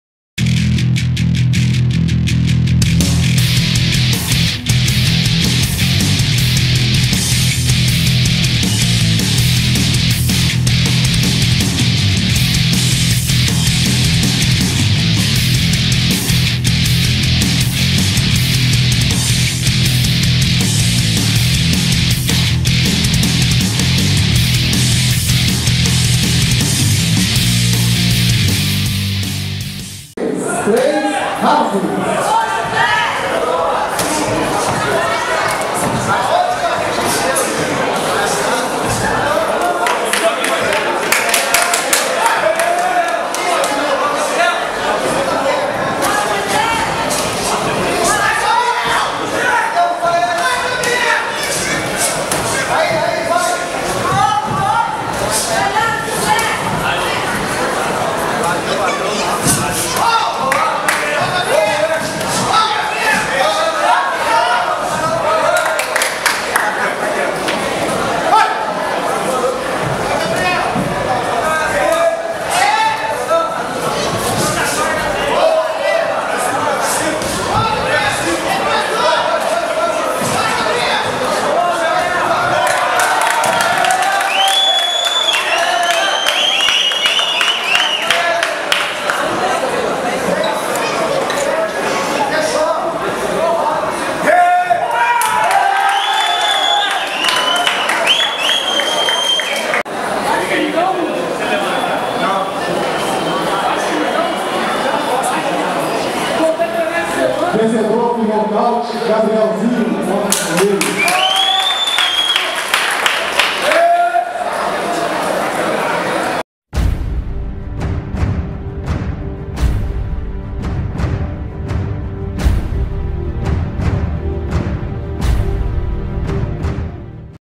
Jose vs Gabriel - BH Sparta 4
Gabrielzinho Rodrigues (Coliseu Araxá) venceu por KO no primeiro round José Fernandez (Combat Clube).